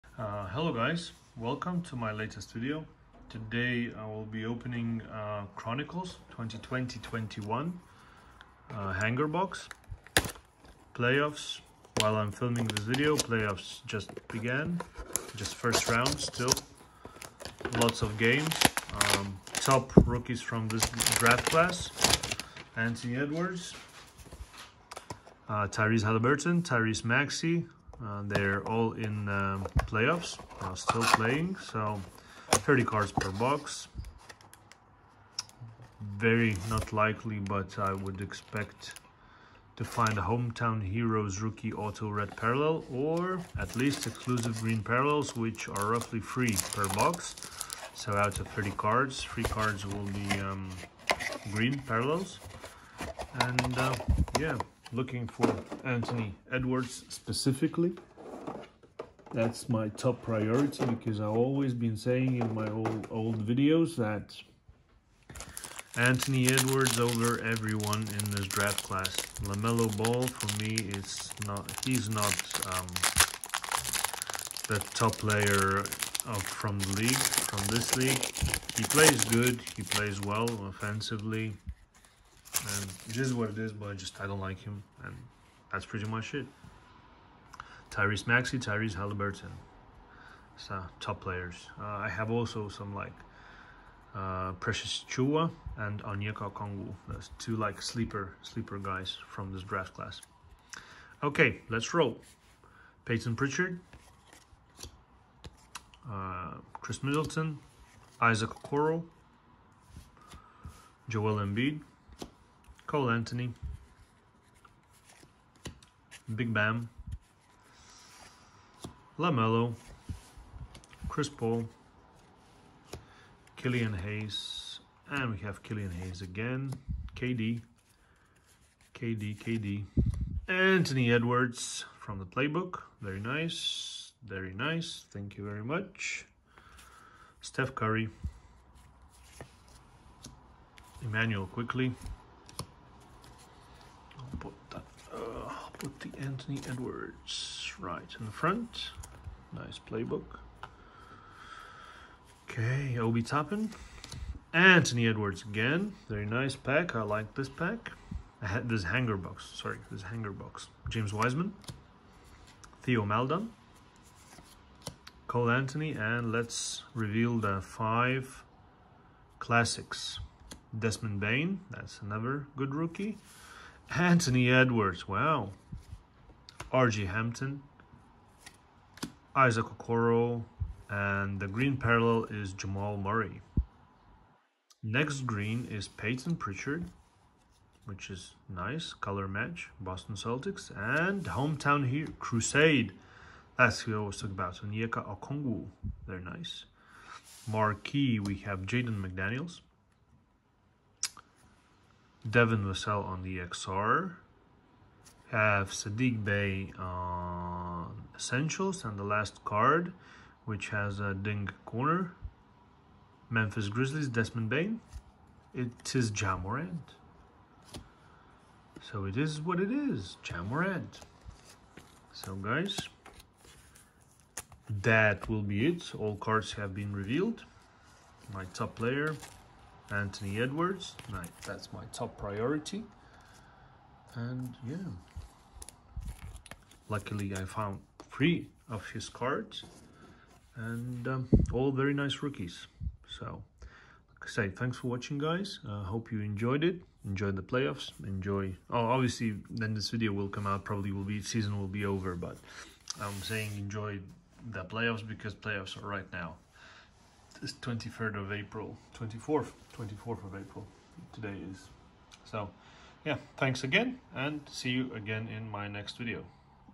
2020-21 Panini Chronicles Basketball Hanger box Opening. Antman Cards! Playoff mode!
2020-21 Panini Chronicles Basketball Hanger box Opening. Multiple Anthony Edwards Rookie cards! Very Good Hanger overall!!! I Enjoyed this hanger opening. Playoff mode - ANTMAN! Lets enjoy this box opening & have fun together!